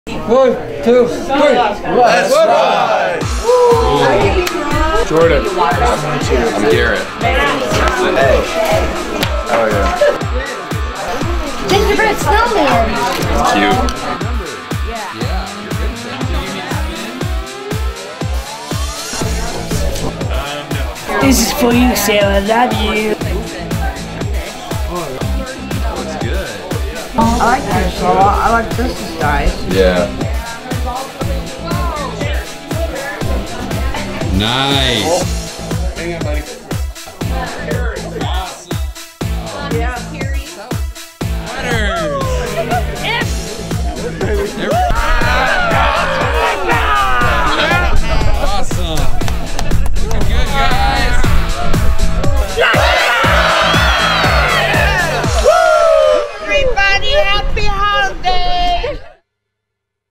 Holiday Sweaters
These Cedar Rapids Rough Riders aren't just crafty on the ice! They show off there craft skills with our Day Habilitation participants by making some festive, holiday sweaters. Check out there fun experience here!